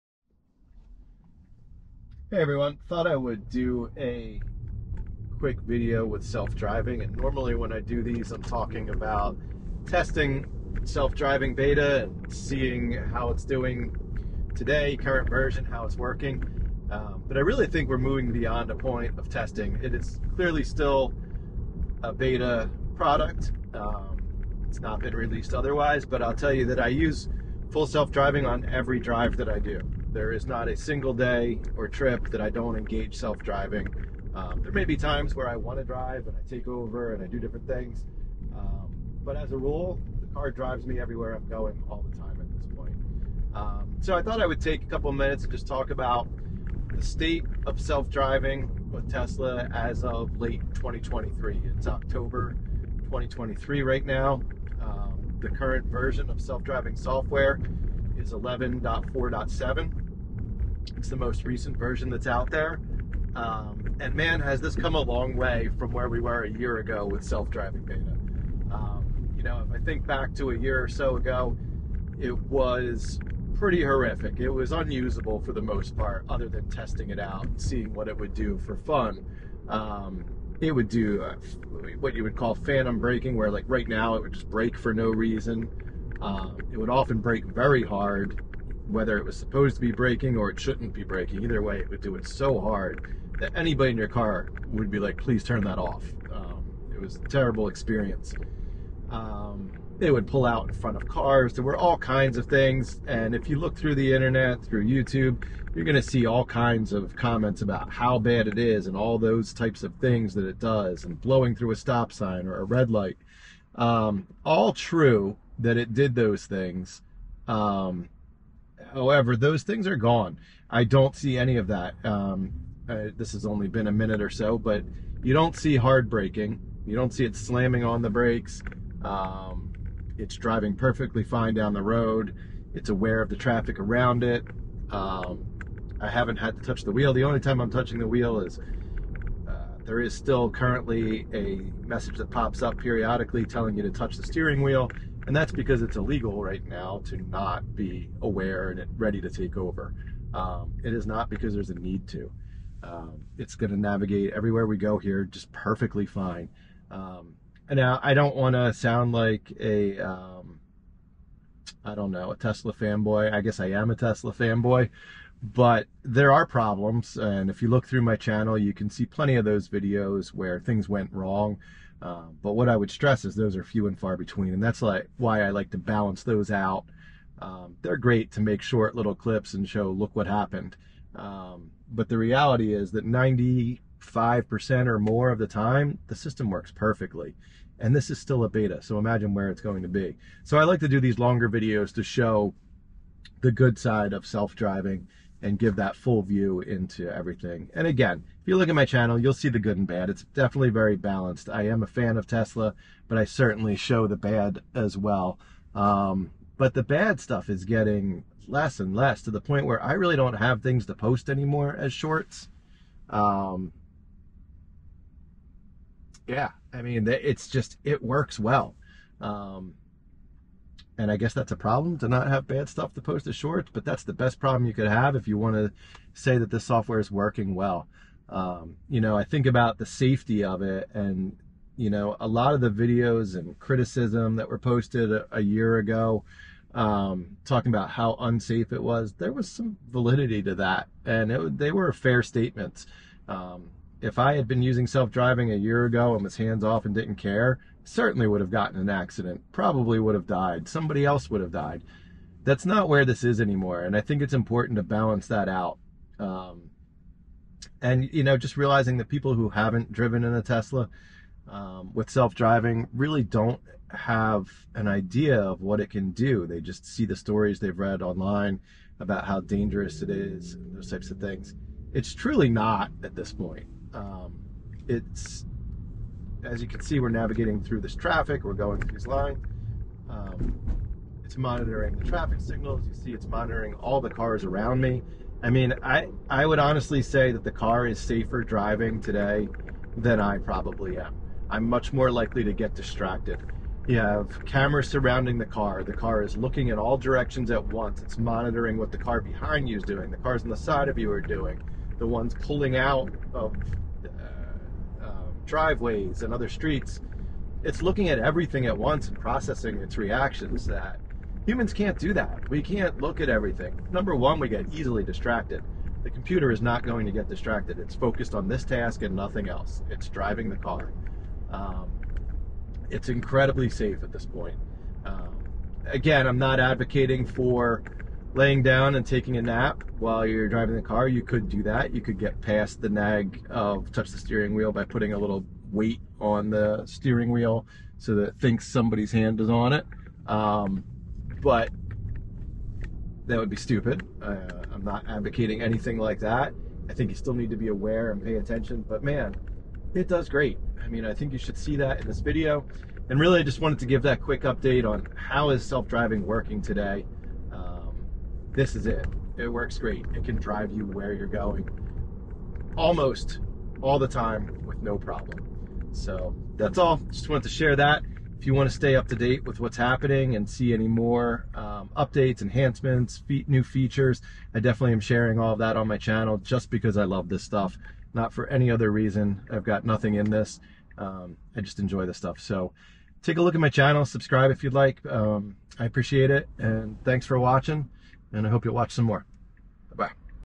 Current State Of Tesla FSD Full Self Driving Late 2023
A look at the performance of Tesla Full Self Driving (FSD Beta) as of late 2023. The improvements over the past year have been amazing. Each iteration is significantly better and better. It won't be much longer before this is 100%. If you have never experienced self driving or if it's been awhile take a look.